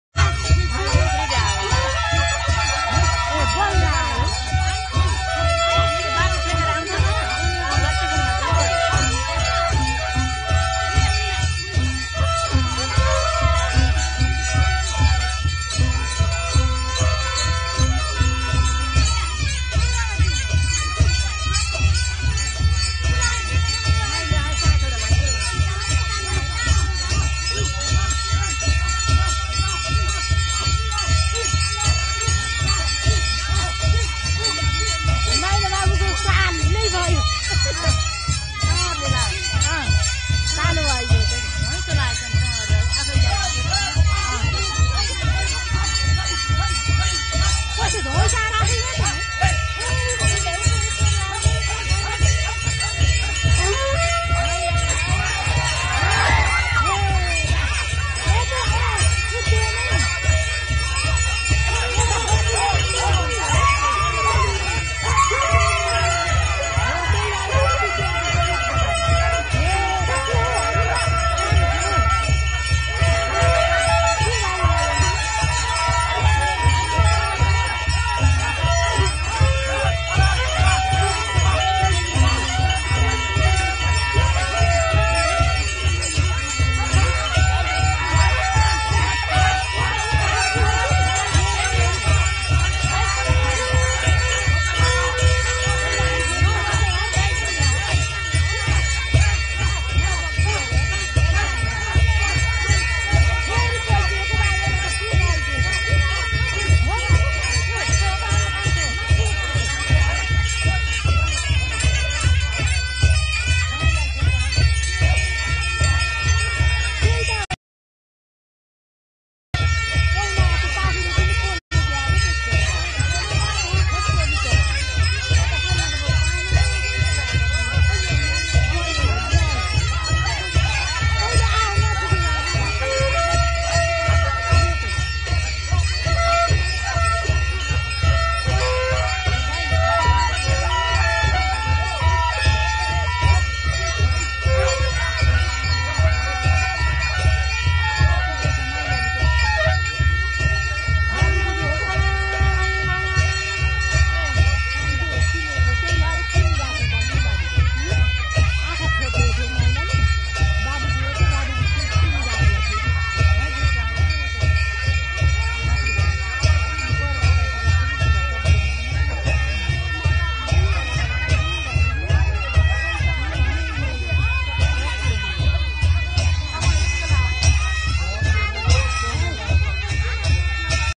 Kulpooja also called Dewali
This video is about typical Nepali family god worship ceremony (Kulpooja also called Dewali)